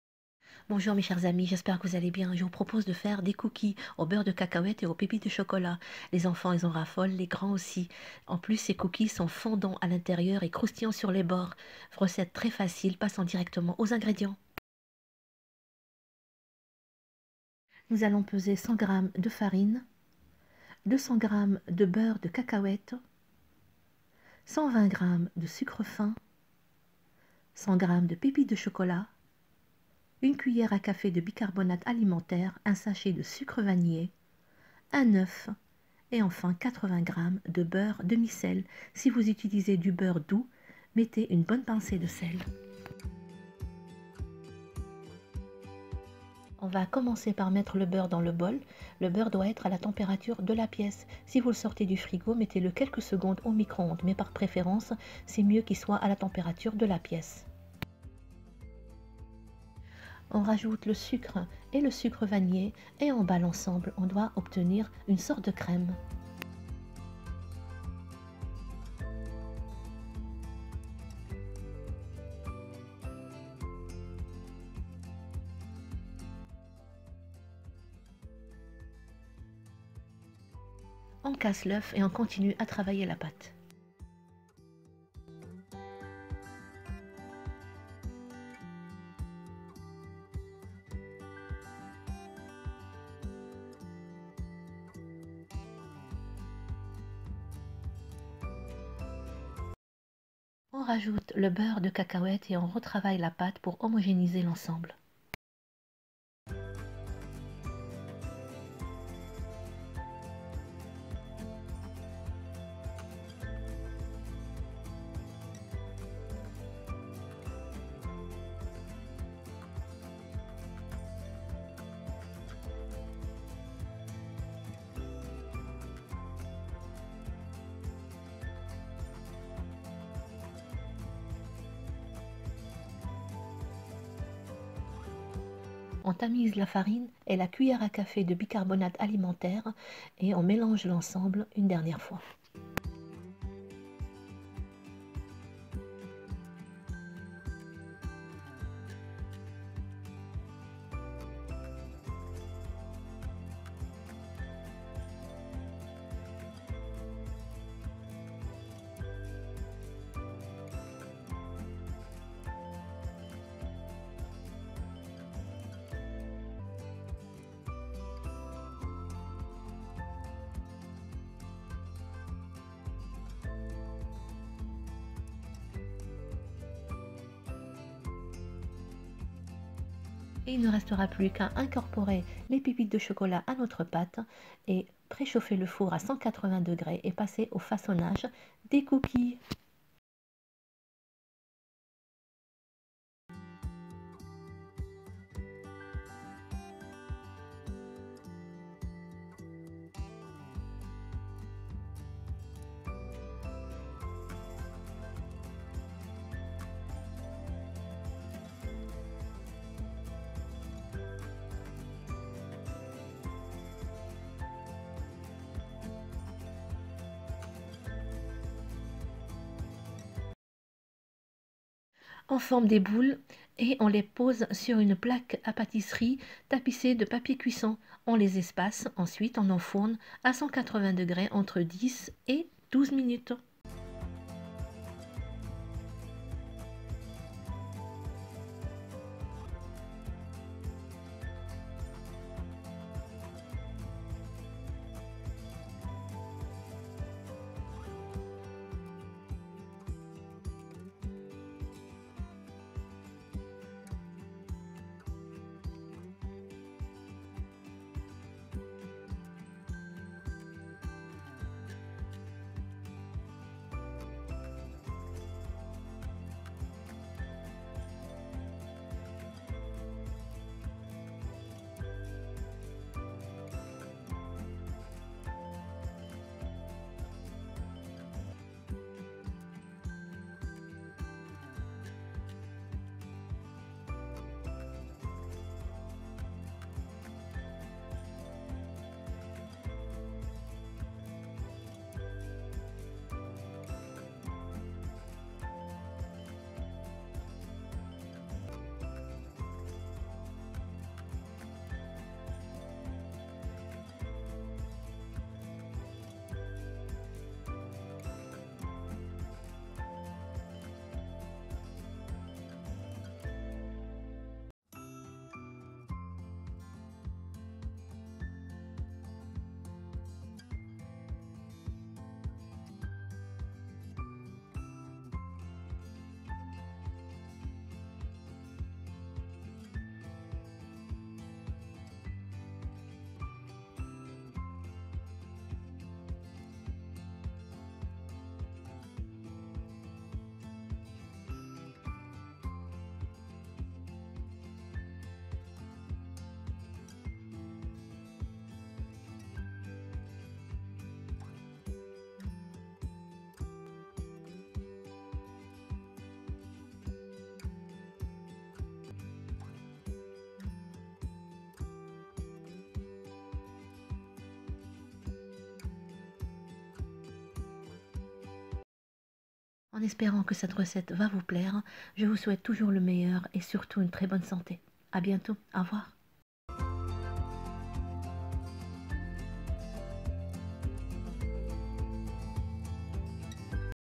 COOKIES BEURRE DE CACAHUÈTES ET PÉPITES DE CHOCOLAT🍪/PEANUT BUTTER AND CHOCOLATE CHIP COOKIES 🍪 🍪
english subtitle/sous-titres en anglais

100g farine
200g beurre de cacahouète
120g sucre
100g pépites de chocolat
80g beurre demi-sel
1c à c bicarbonate alimentaire
1 sachet sucre vanillé
1 œuf

100 g flour
200g peanut butter
120g sugar
100g chocolate chips
80g semi-salted butter
1 tsp food baking soda
1 sachet vanilla sugar
1 egg